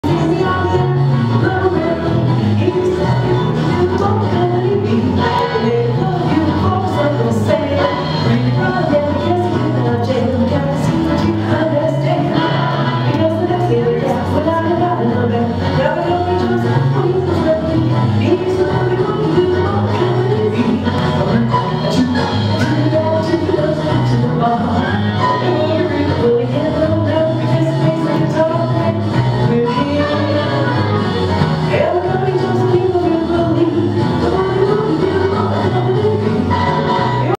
Lynne Singing At Mansion Alcazar
Lynne Nadeau from Canada performing her first public concert at Mansion Alcazar.  This video was taken Sept 8/16  in Cuenca, Ecuador.